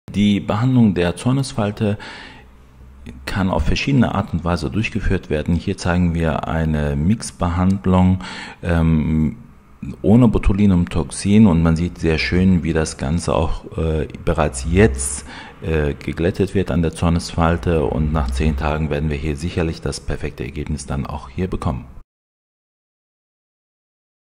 Mc Aesthetics präsentiert die Zornesfalte
Esin B. aus Dortmund schreibt:,, Meine Zornesfalte ist endlich weg.
Ich habe mir ganz oft anhören müssen, das ich schlecht gelaunt aussehe oder immer so böse schaue, obwohl das garnicht der Fall ist. Mit meinem ersten Ergebnis bin ich jetzt schon sehr zufrieden un dich komme auf jeden Fall wieder, es war toll bei euch.
Danke Mc Aesthetics.''